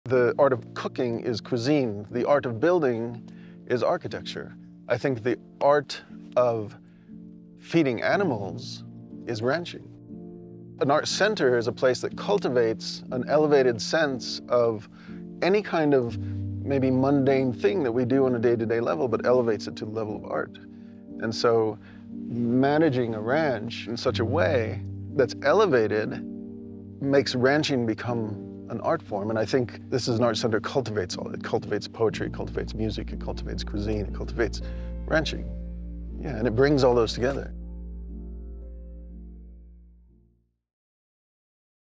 Relevance of Place - Group - What Is
“An art center is a place that cultivates an elevated sense of any kind of maybe mundane thing that we do on a day-to-day level, but elevates it to the level of art…And I think this as an art center, cultivates all of it.  It cultivates poetry.  It cultivates music. It cultivates cuisine. It cultivates ranching.  And it brings all those together.”
- Ronald Rael